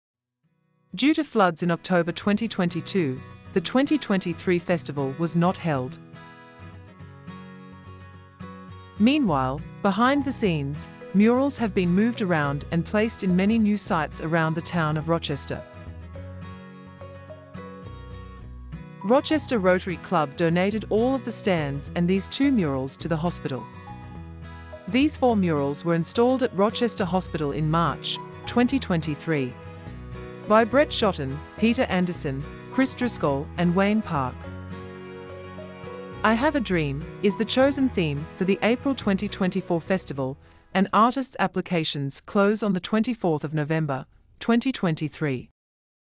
2024 Rochester Mural Festival
Our region was damaged by floods in October 2022. The 2023 Rochester Mural Festival could not be held.

Rochester's strong and resilient community is planning for its 2024 Rochester Mural Festival with the theme, "I Have a Dream". Mural artists please apply at rochestermuralfest.com.au